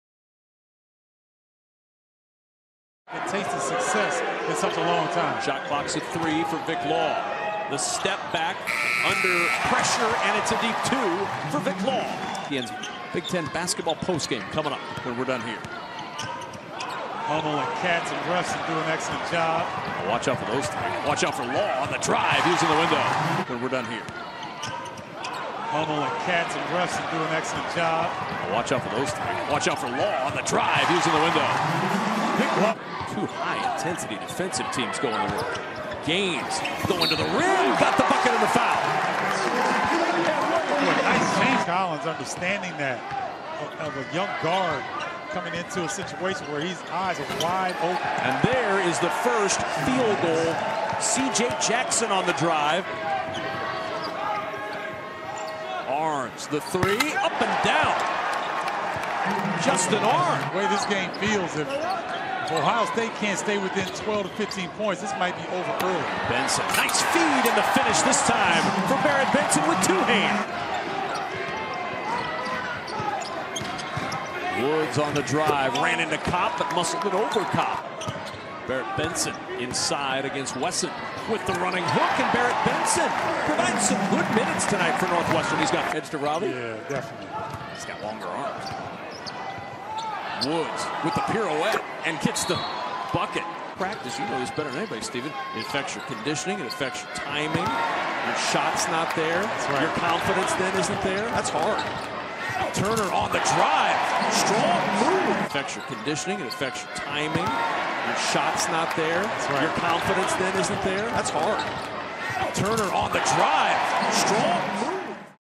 First Half Highlights: Ohio State vs. Northwestern | B1G Basketball
The top plays of the first half.